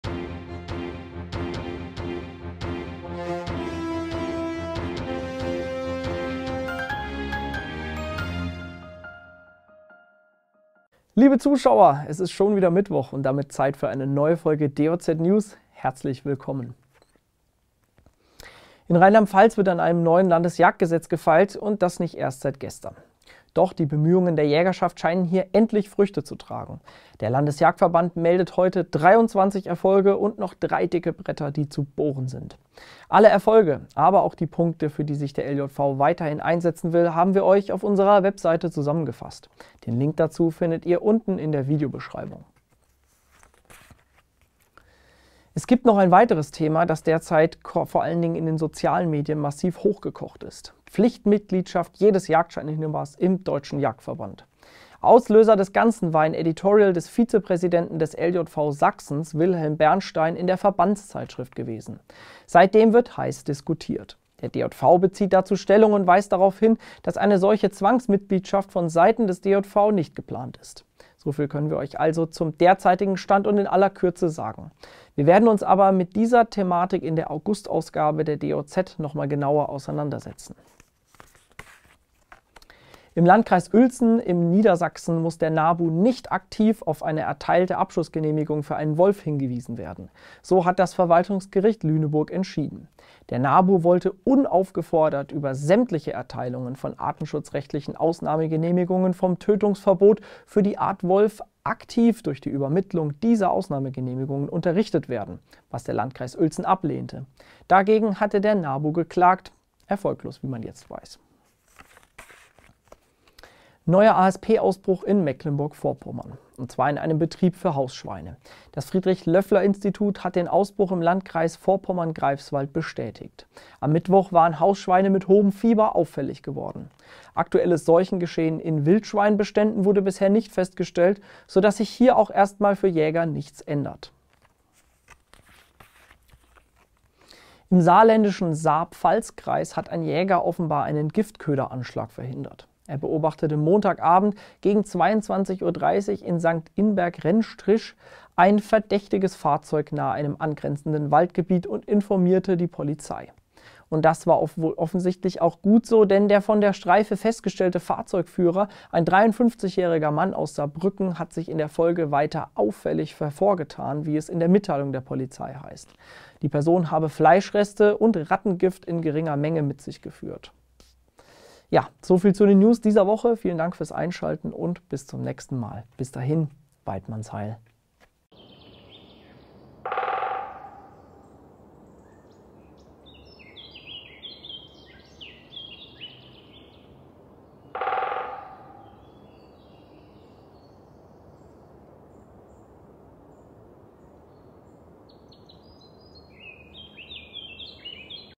DJZ News 12.06.2024
In Rheinland-Pfalz wird an einem neuen Landesjagdgesetz gefeilt.

Es gibt noch ein weiteres Thema, das kürzlich vor allem in den sozialen Medien massiv hochgekocht ist: Pflichtmitgliedschaft jedes Jagdscheininhabers im Deutschen Jagdverband.

Der Landkreis Uelzen in Niedersachsen muss den NABU nicht „aktiv“ auf eine erteilte Abschussgenehmigung für einen Wolf hinweisen.

Neuer ASP-Ausbruch in Mecklenburg-Vorpommern.

Im saarländischen Saarpfalz-Kreis hat ein Jäger offenbar einen Giftköderanschlag verhindert.